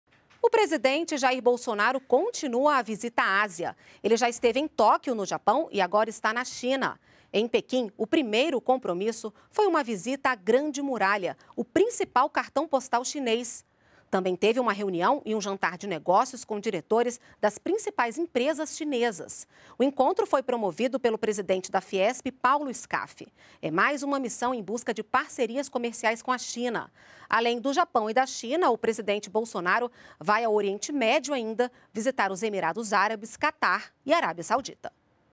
Presidente Bolsonaro visita a Muralha da China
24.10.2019 - Em Pequim, o primeiro compromisso foi uma visita à Grande Muralha, o principal cartão postal chinês.Também teve uma reunião e um jantar de negócios com diretores das principais empresas chinesas.